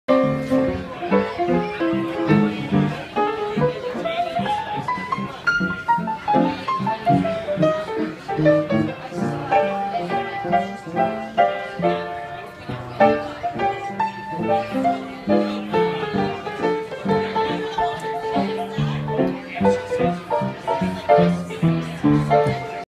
Steve Hirsch on piano
Piano player, Bull Run Restaurant, Shirley, MA Oct 2015